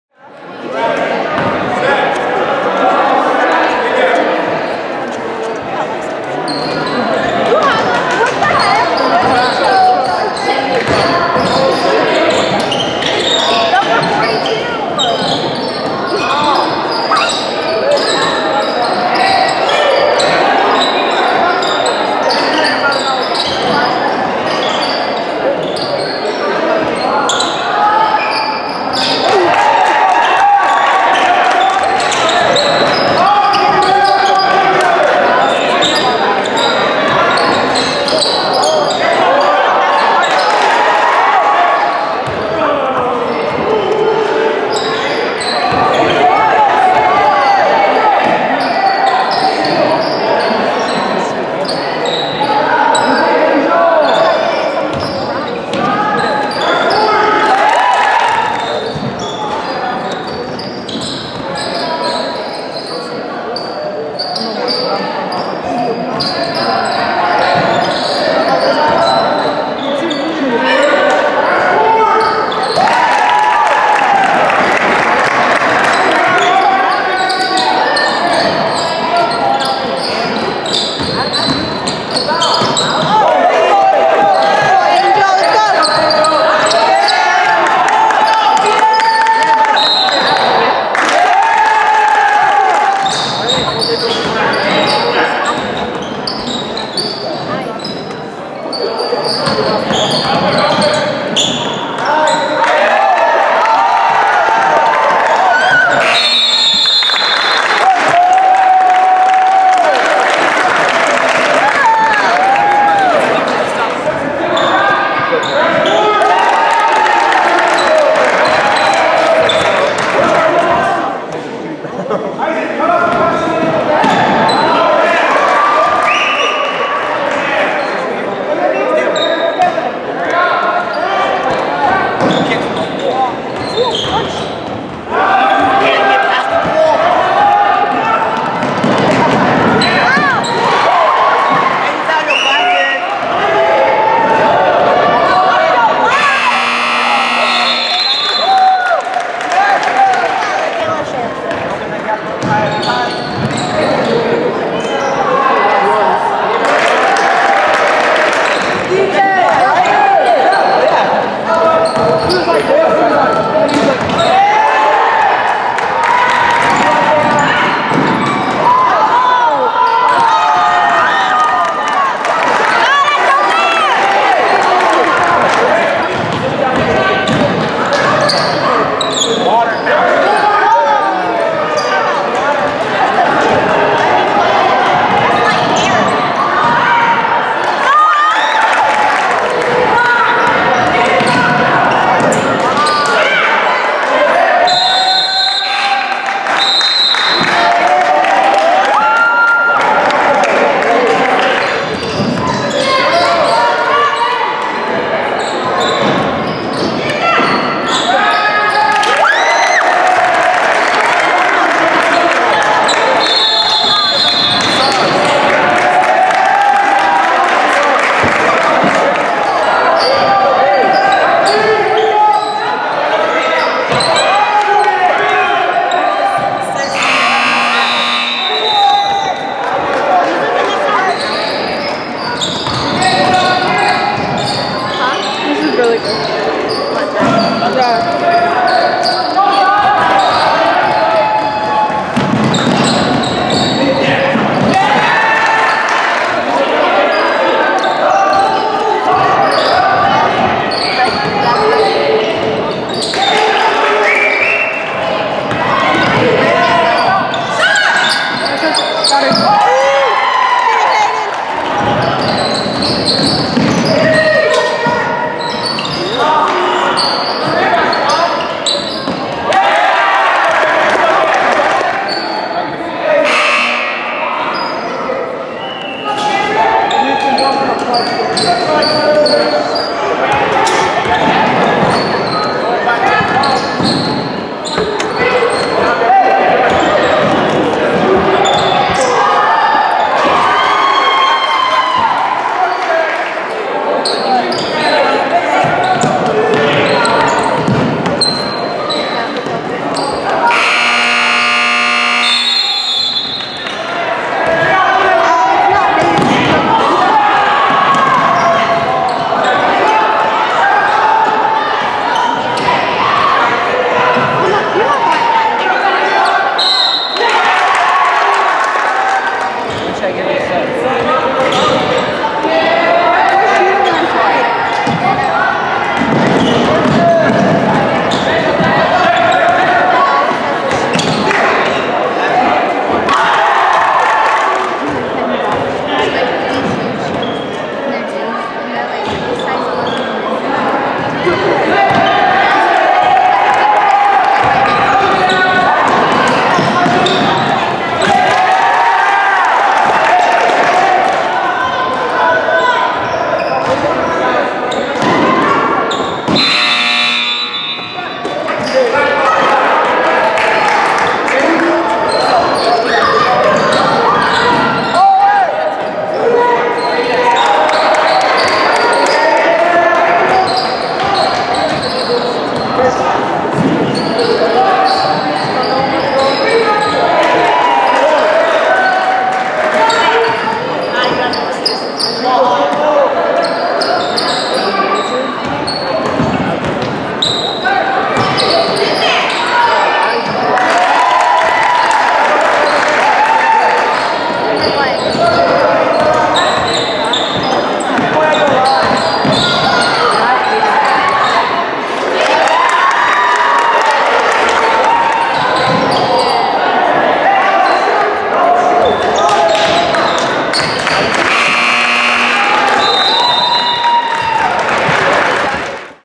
Hanshew Hawks at Mears Panthers - 2017 ASD JV Middle School League
The highlights from the match between Mears & Hanshew middle school JV Basketball teams.  Mears middle school is 0-2 and in black and Hanshew middle school is 1-1 and in white.  This regular season game took place at Mears middle school on February 14, 2017.